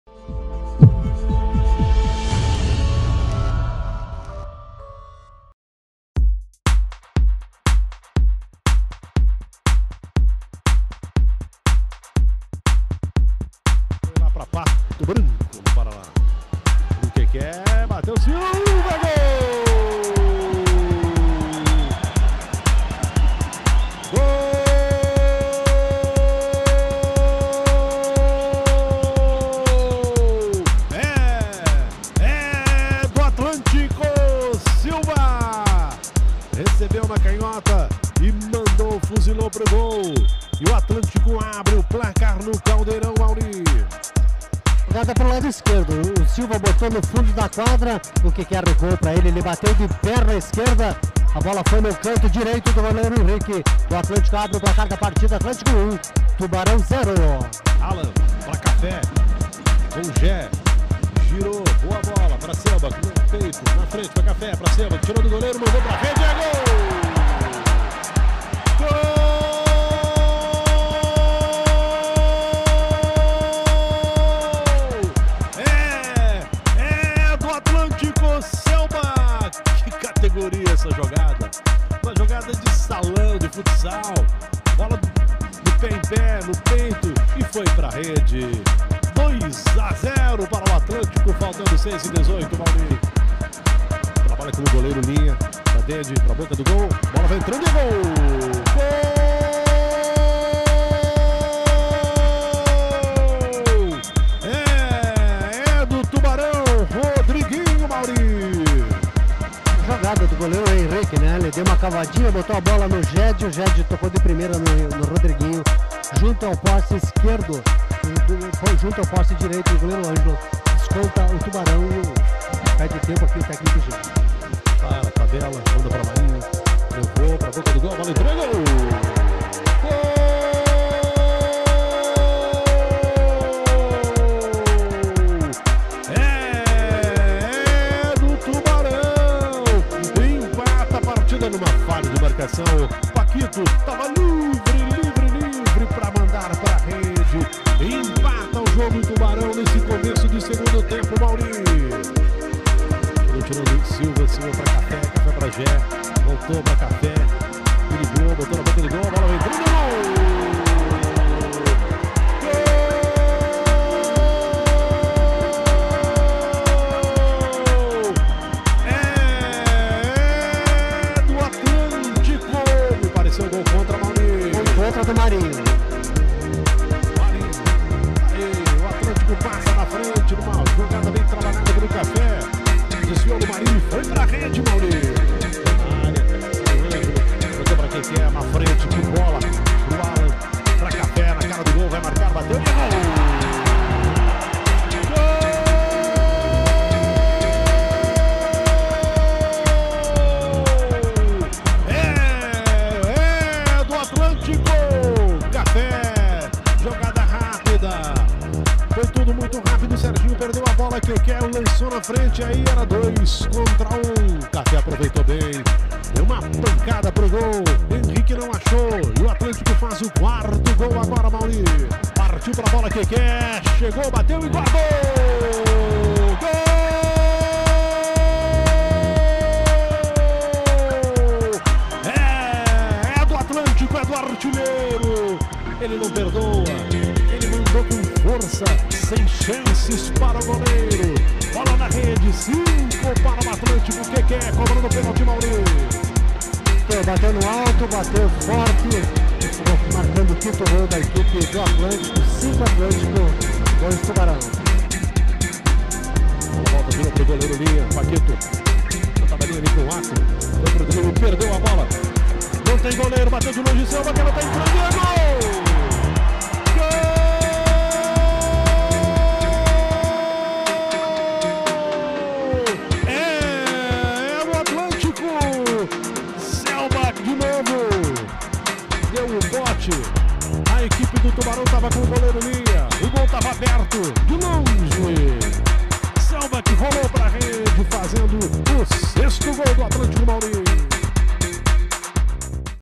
LNF2018 - Atlântico 6 x 2 Tubarão - Gols - 15ª Rodada
Gols:
Atlântico - Silva, Café, Keké, Marinho (contra) e Selbach (2)
Tubarão - Rodriguinho e Pakito